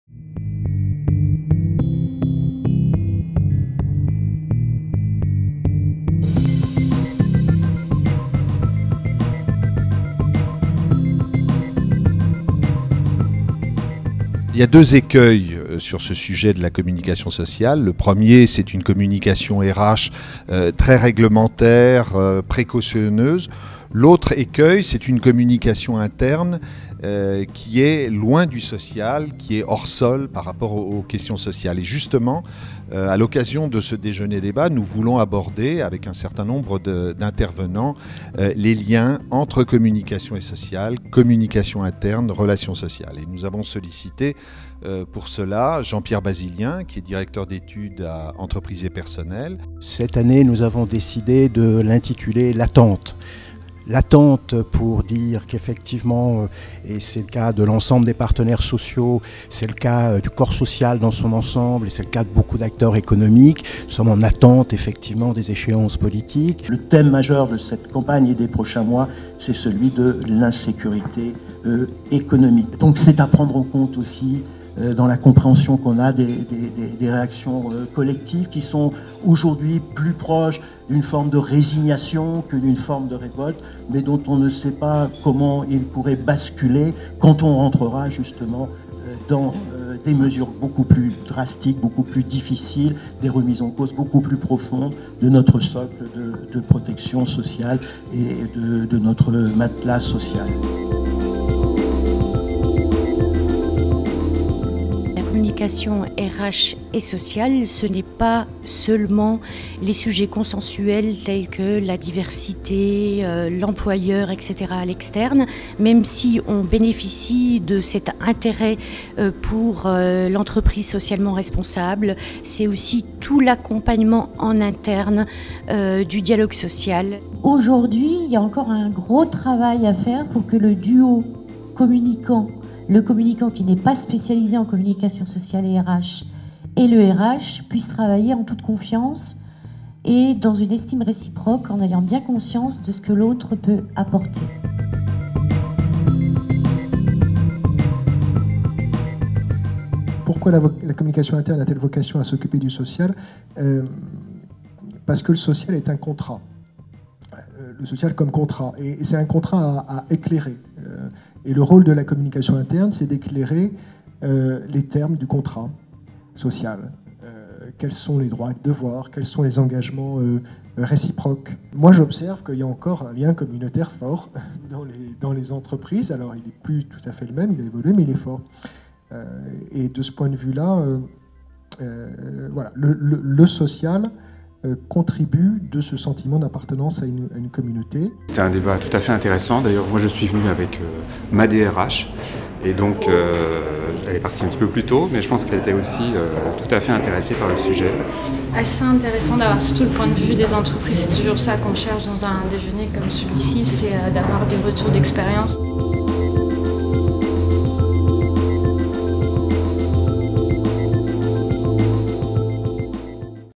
Déjeuner-débat Afci "Communication interne et relations sociales"
L'Afci a organisé, le 14 février 2012,  un déjeuner-débat sur le thème suivant : "communication interne et relations sociales : collaboration impossible ou nouvelle donne ?"

Les organisations, qu'elles soient grandes ou petites, auront besoin en 2012 de développer une communication sur les questions sociales. Mais quelle communication au juste ? Les salariés attendent cohérence, précision et pédagogie. Les entreprises devront éviter notamment l'écueil d'une communication à deux vitesses, avec d'un côté une communication sociale très « réglementaire »  respectant à la lettre les textes juridiques et ne créant pas réellement les conditions de l'écoute et du dialogue et de l'autre une communication interne indexée sur la seule stratégie globale de l'entreprise et de fait souvent désocialisée.

Comment, dans une période difficile, penser dans des termes nouveaux communication interne et communication sociale ? Quels rapprochements ? Quels modes de collaboration ? Quelles passerelles ?

Nous avons échangé autour de ces questions avec :

- Jean-Pierre Basilien, directeur d'études au sein d'Entreprise & Personnel.
- Christine Blanc-Michelland, responsable de la communication sociale et RH d'ERDF.
- Thierry Garnier, responsable de la communication interne de Renault Trucks, administrateur de l'Afci.

Ce débat était animé par Jean-Marie Charpentier, secrétaire général de l'Afci et responsable de l'observation sociale et des études d'ERDF.